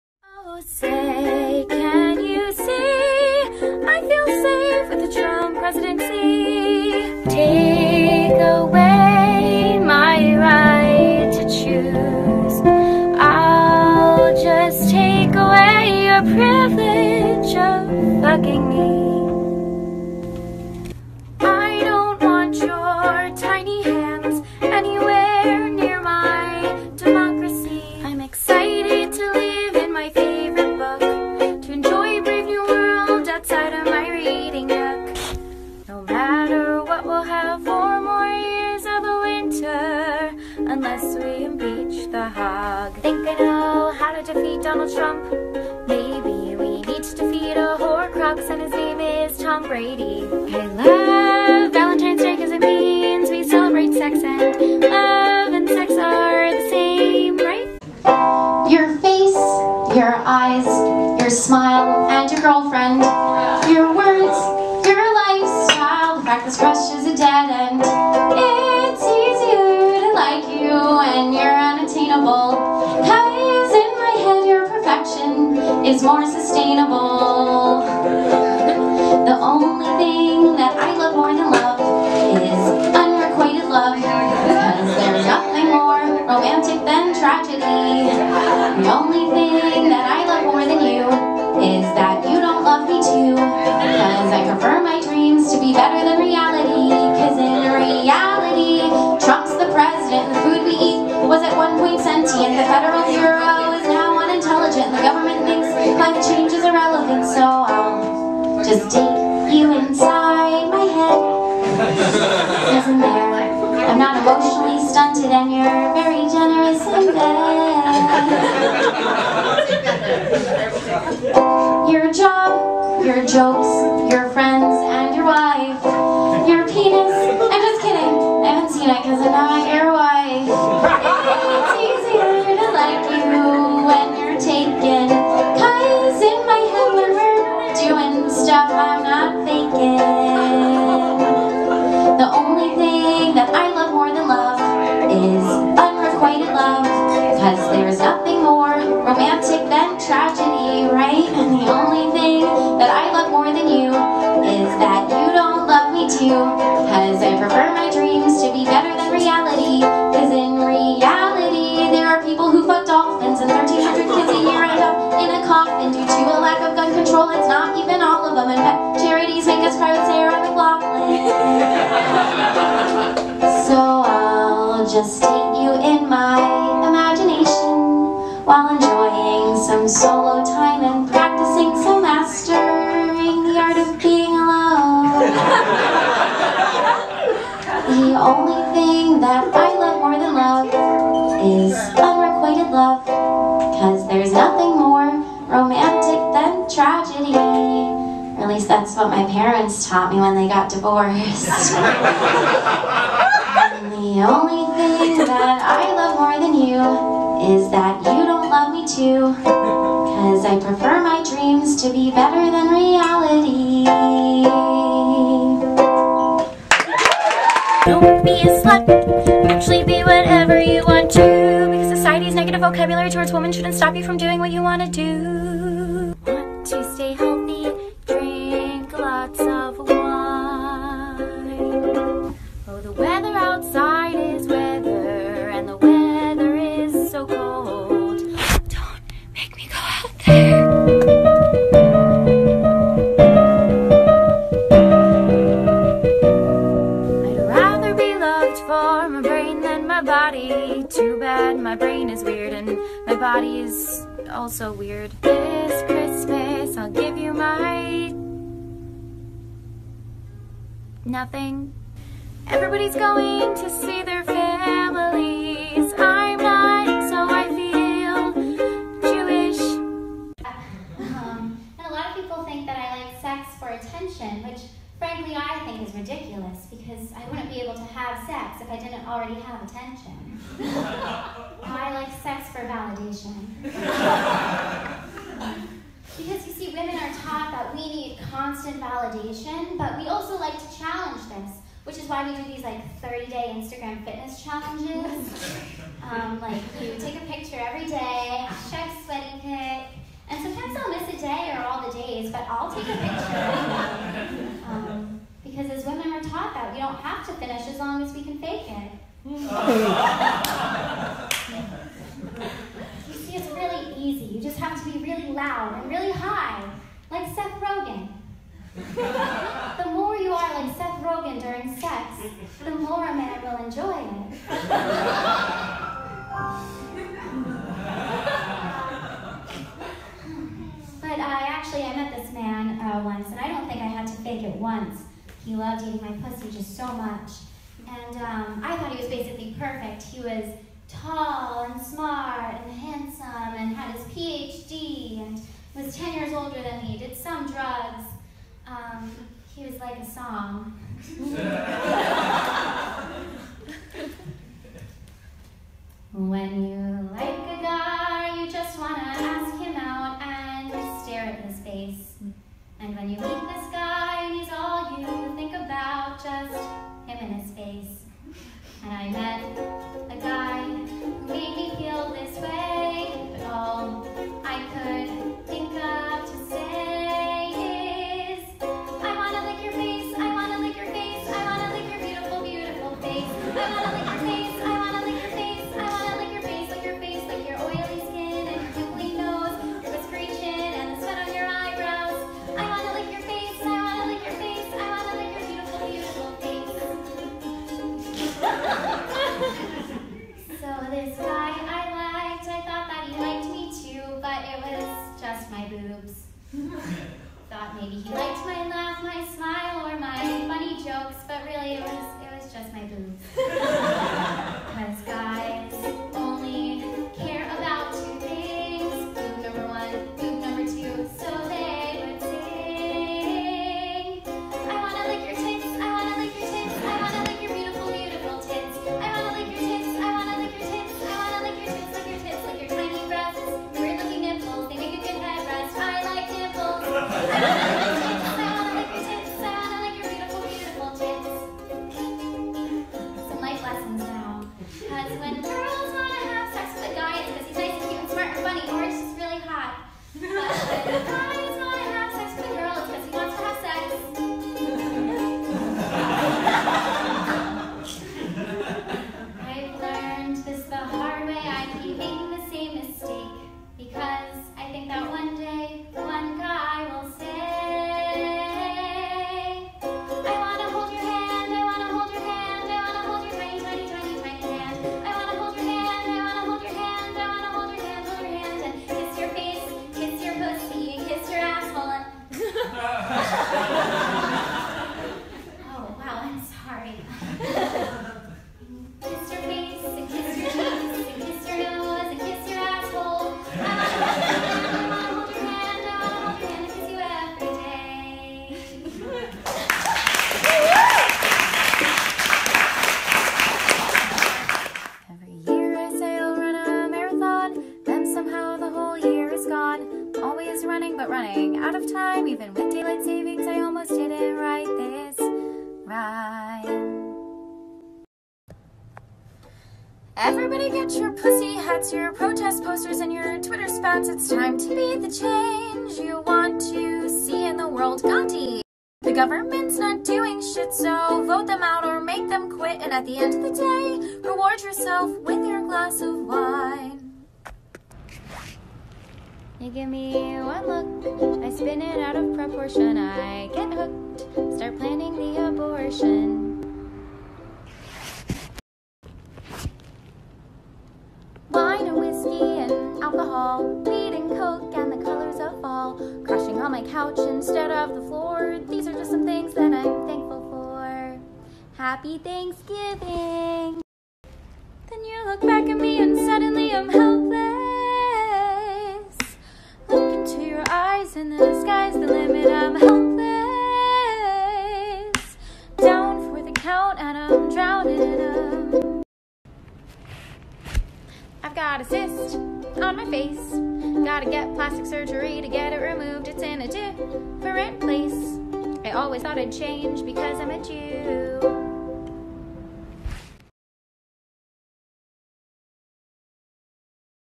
Jamie Lerner Music Comedy Reel (Social Media, 2020)
A poorly self-edited compilation of micro songs I call Jam a Days, some live performances of songs, and some stand-up from the various years.